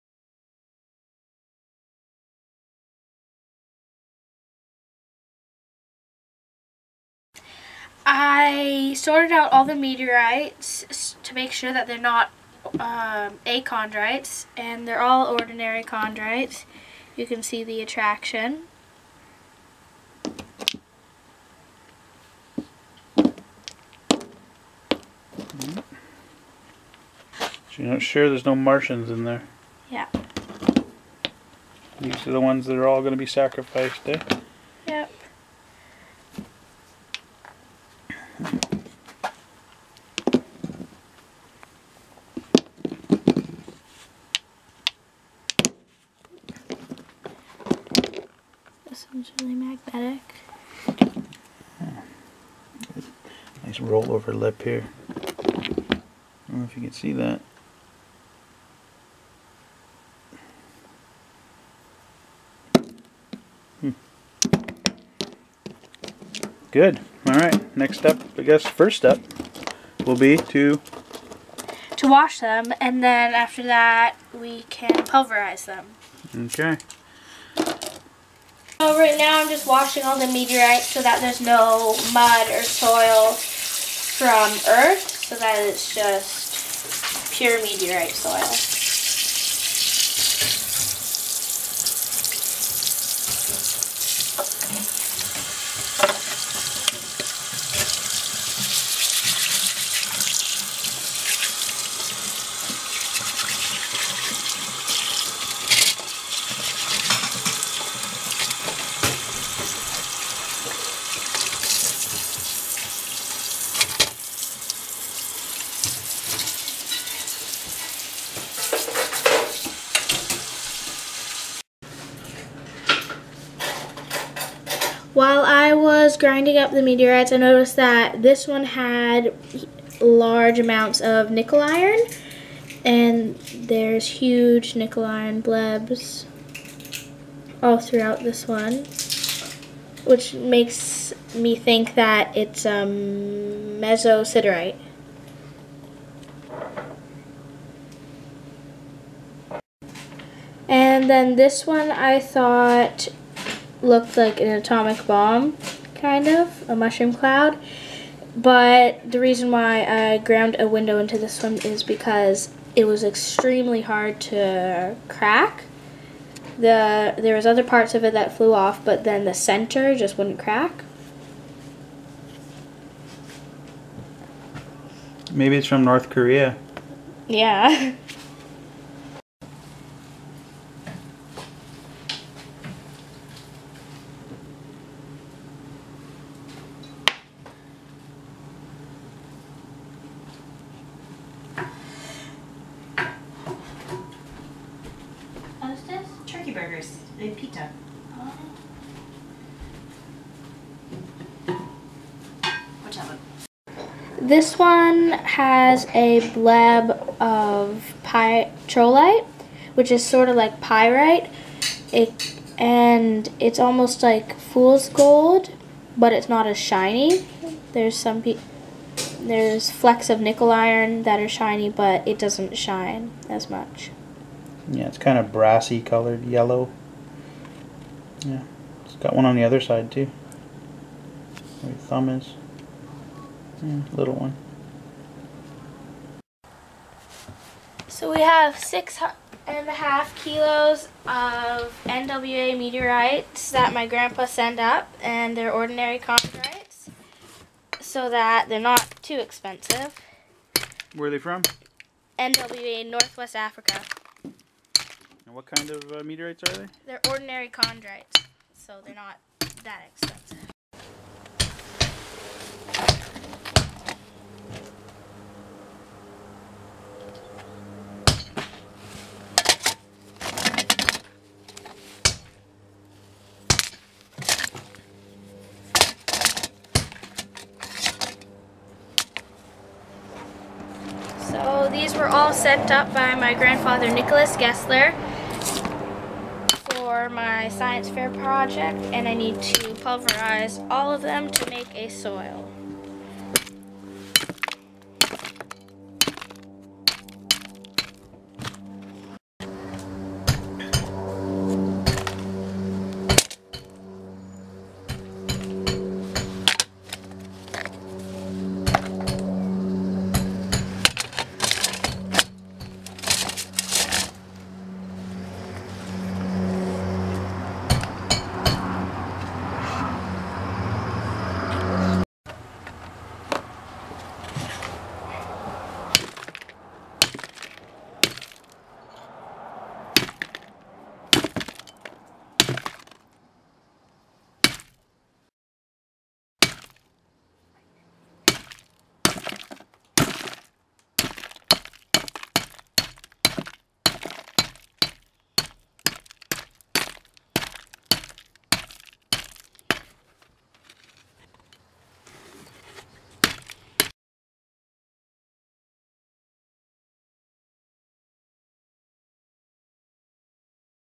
METEORITE SOIL or destroying perfectly good meteorites
Step one, pulverize 6.5 kilograms of meteorites.
This is the first stage of a science fair project.